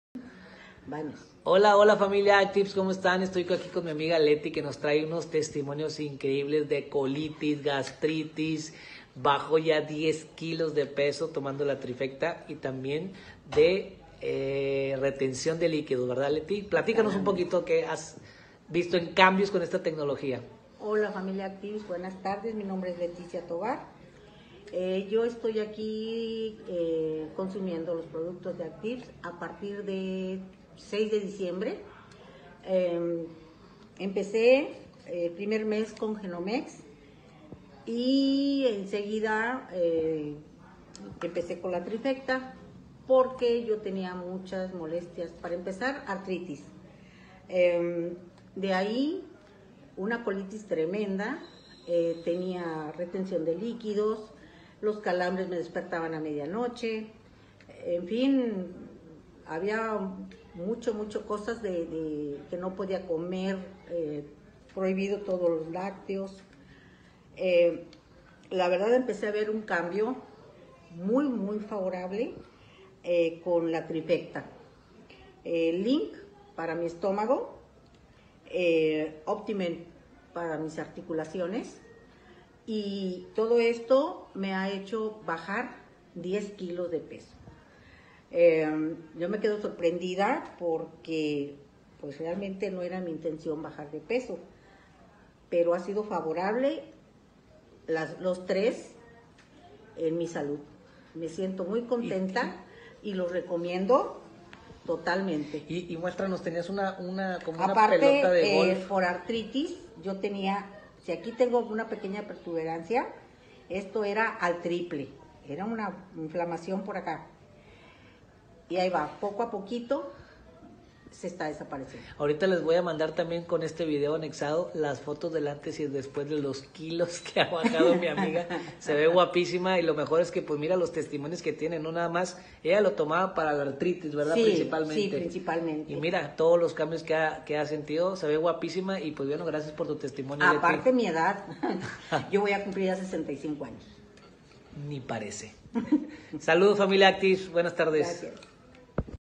colitis, retención de líquidos, y gnmx. testimonio
RETENCIÓN DE LÍQUIDOS
La retención de líquidos, es la hinchazón causada por la acumulación anormal de líquidos en el cuerpo. El líquido se acumula bajo la piel, dentro de los tejidos que están fuera del sistema circulatorio.

🟠 Conoce y dale la oportunidad a nuestros productos